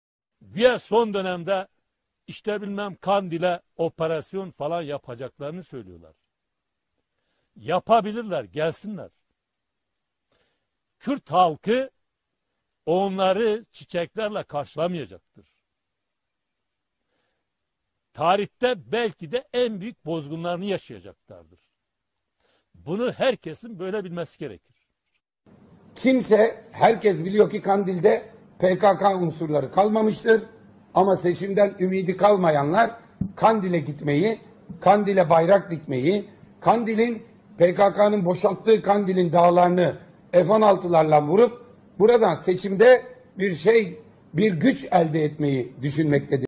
Cemil Bayık,Özgür Özel'i yalanlıyor !!!(Video alıntıdır)
CHP'li Özgür Özel,dün yaptığı açıklamada Kandil'e yapılacak operasyonun Ak partinin seçimi kazanmaya yönelik atacağı bir adım olduğunu,Kandil'de teröristin kalmadığını,dağın taşın bombalanacağını söylemişti.Anlaşılan o ki Özgür Özel,Pkk elebaşlarından Cemil Bayık'ı terörist olarak görmüyor.Kandil'in PKK kurulduğundan beri teröristlerin sığındığı bir liman olduğundan,PKK'lıların  buralarda her türlü eğitilip kamplar kurduklarını